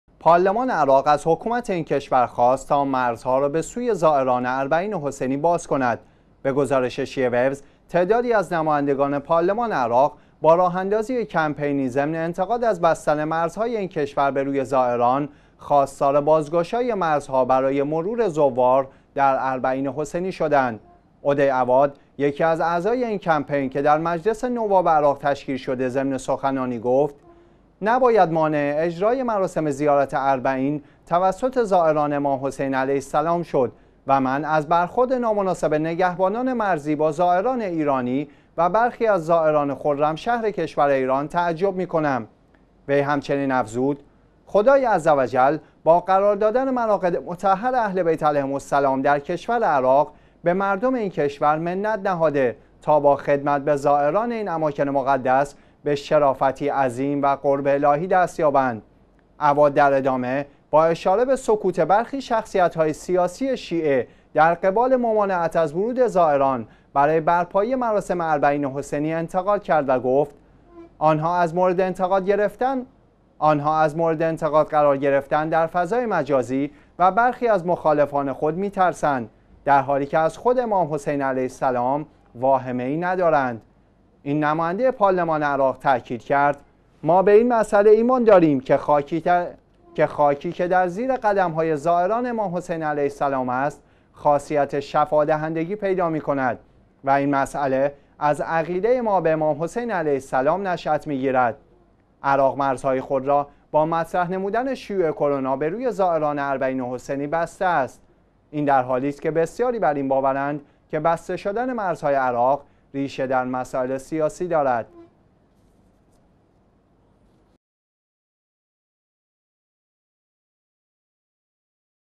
درخواست نمایندگان عراقی برای بازگشایی مرزها به روی زائران اربعین حسینی
پارلمان عراق از حكومت این کشور خواست تا مرزها را به سوی زائران اربعین حسینی باز کند.
به گزاش شیعه ویوز، تعدادی از نمایندگان پارلمان عراق با راه اندازی کمپینی ضمن انتقاد از بستن مرزهای این کشور به روی زائران، خواستار بازگشایی مرزها برای مرود زوار در اربعین حسینی شدند.
عدی عواد، یکی از اعضای این کمپین که در مجلس نواب عراق تشکیل شده، ضمن سخنانی گفت: نباید مانع اجرای مراسم زیارت اربعین توسط زائران امام حسین علیه السلام شد و من از برخورد نامناسب نگهبانان مرزی با زائران ایرانی و برخی از زائران خرمشهر کشور ایران تعجب می کنم.
وی همچنین افزود: خدای عزوجل با قرار دادن مراقد مطهر اهلبیت علیهم السلام در کشور عراق به مردم این کشور منت نهاده تا با خدمت به زائران این اماکن مقدس به شرافتی عظیم و قرب الهی دست یابند.
عواد در ادامه با اشاره به سکوت برخی شخصیت های سیاسی شیعه در قبال ممانعت از ورود زائران برای برپایی مراسم اربعین حسینی انتقاد کرد و گفت: آنها از مورد انتقاد قرار گرفتن در فضای مجازی و برخی مخالفان خود میترسند درحالی که از خود امام حسین علیه السلام واهمه ای ندارند.
این نماینده پارلمان عراق تاکید کرد: ما به این مسأله ایمان داریم که خاکی که در زیر قدم‌های زائران امام حسین علیه السلام است، خاصیت شفا دهندگی پیدا می‌کند و این مسأله از عقیده ما به امام حسین علیه السلام نشأت می گیرد.
عراق مرزهای خود را با مطرح نمودن شیوع کرونا به روی زائران اربعین حسینی بسته است.
این درحالی است که بسیاری بر این باورند که بسته شدن مرزهای عراق ریشه در مسائل سیاسی دارد.